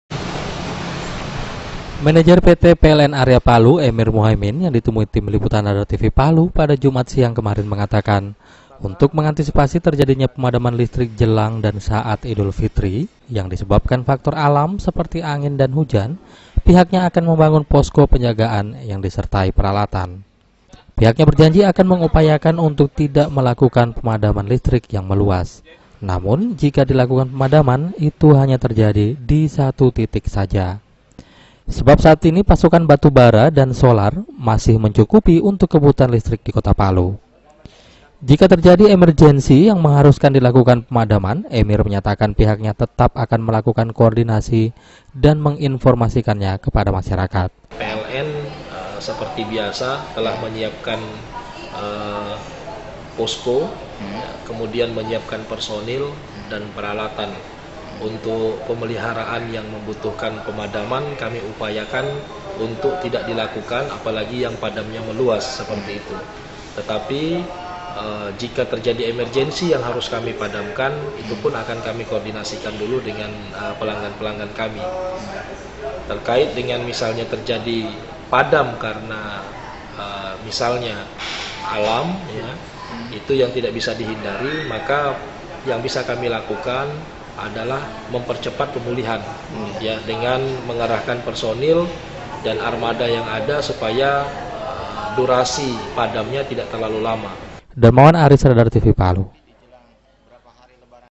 PLN Area Palu Upayakan Tidak Terjadi Pemadaman Listrik Jelang dan Saat Idul Fitri
Untuk mengantisipasi terjadinya pemadaman listrik menjelang dan saat Idul Fitri. PT. PLN  Area Palu, akan menyiagakan petugas dan membuat posko yang dapat menjadi tim tanggap saat terjadi pemadaman.